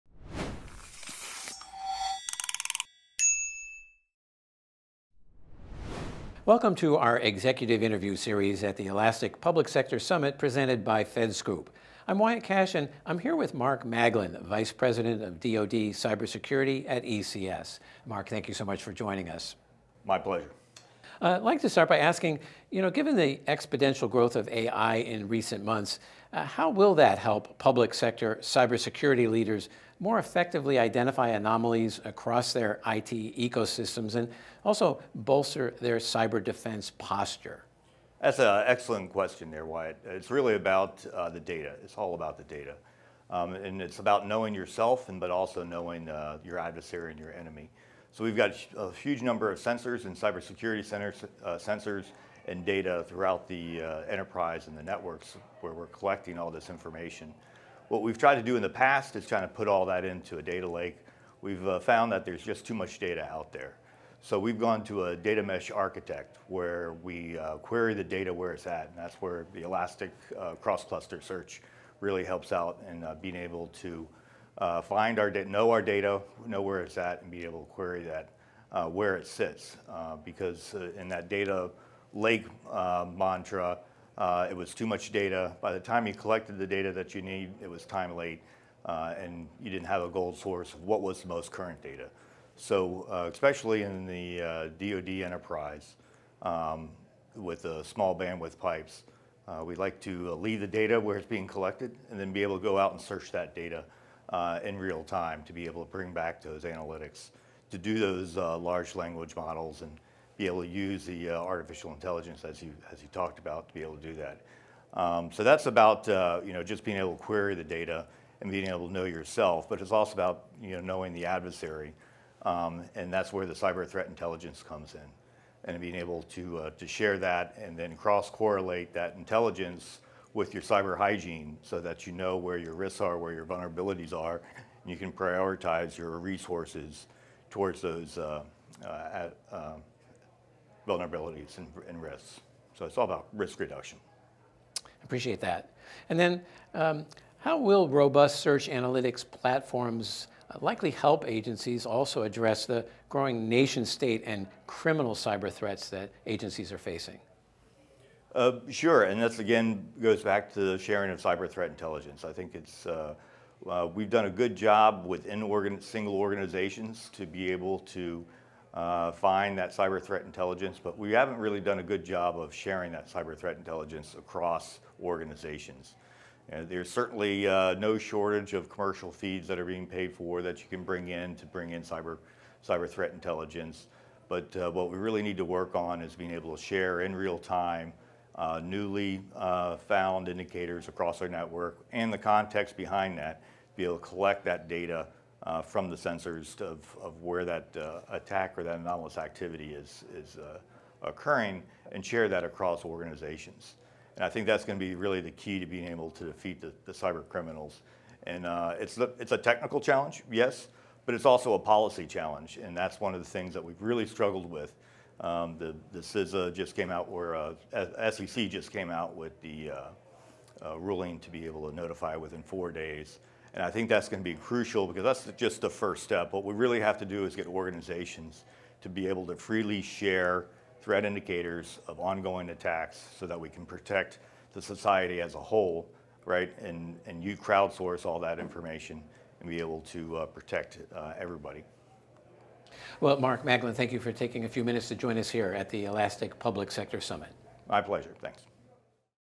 Mark Maglin, ECS | Elastic Public Sector Summit 2024
Government agencies are confronting a new era of challenges in extracting value from their data. As the volume and velocity of data from mission and operations, cybersecurity systems, IoT and edge devices, cloud application traces, and now generative AI keep growing, agency leaders need more effective ways to capitalize on the value of all that data.

Mark Maglin, VP, DOD Security, at ECS, joined top federal program executives and IT leaders at the Elastic Public Sector Summit 2024 to learn firsthand how advances in data management, search, and analytics capabilities are helping agencies turn data into mission value faster and more productively for citizens and government employees.

In this interview, Maglin answered the following questions:
1. Given the exponential growth of AI in recent months, how will that help public sector cybersecurity leaders more effectively identify anomalies across their IT ecosystems and bolster their cyber defense posture?
2. How will robust search analytics platforms likely help agencies address growing nation-state and criminal cyber threats and better protect critical infrastructure?

This interview is part of a series filmed in conjunction with the Elastic Public Sector Summit 2024 held on March 13, 2024, in Arlington, V.A.